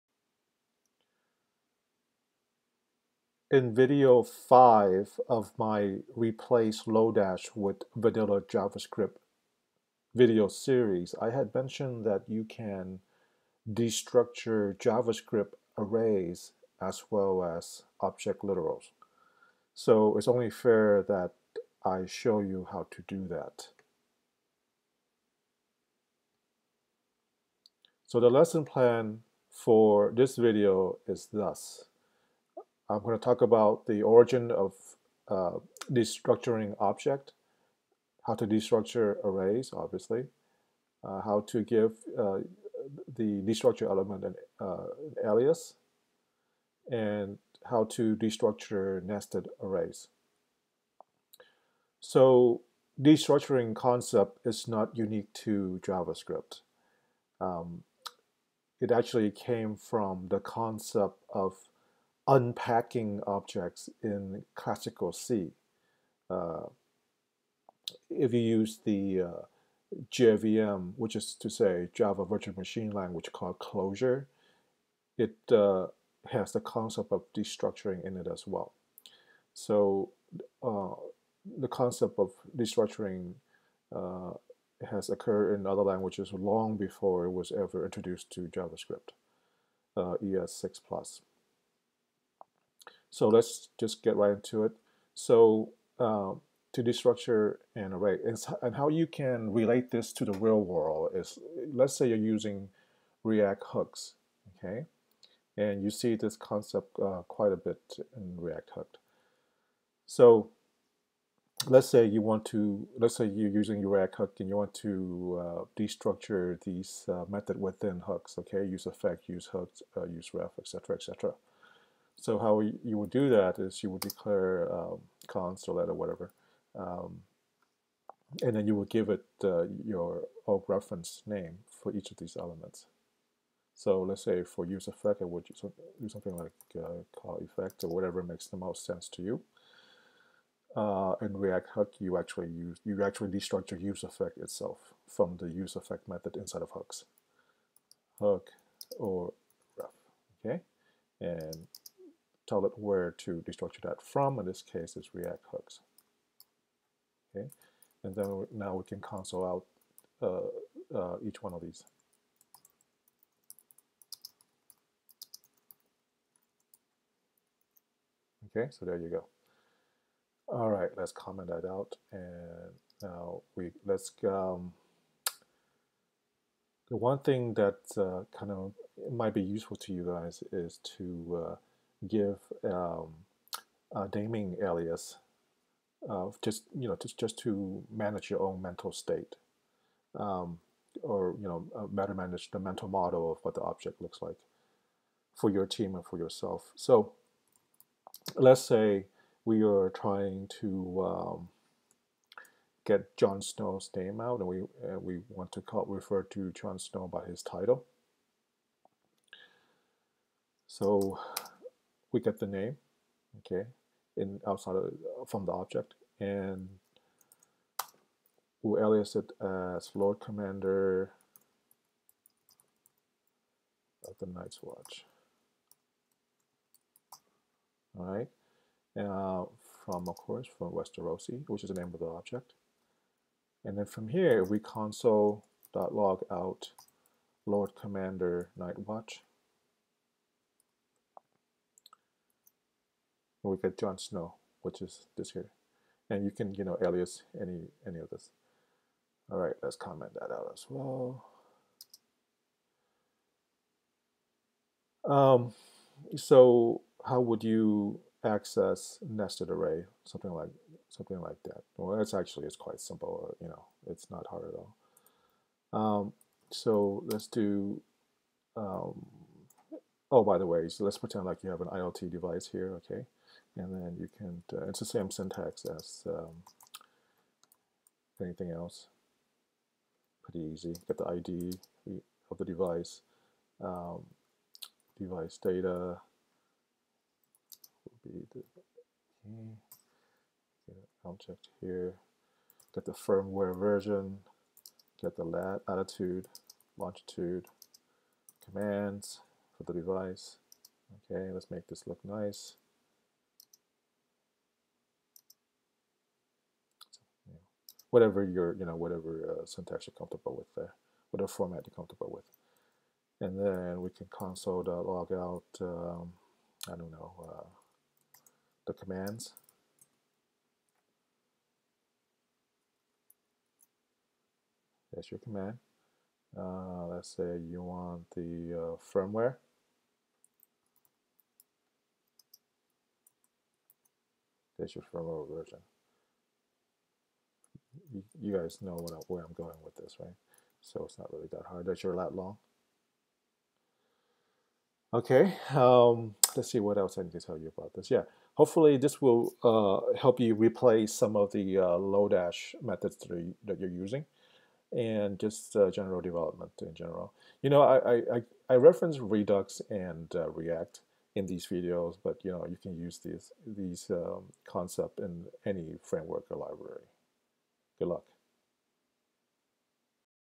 Replace Lodash With Vanilla JavaScript Part 5.1 Destructuring Arrays
How to destructure JavaScript arrays without using external libraries.  MDN documentation of JavaScript destructuring